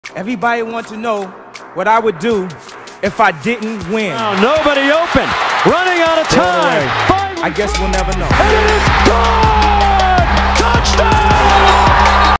X League | 2022 Season | The Legend of KK Matheny
There were certainly a lot of offensive fireworks in the 2022 season of the X League led by the Seattle Thunder and their QB1 KK Matheny.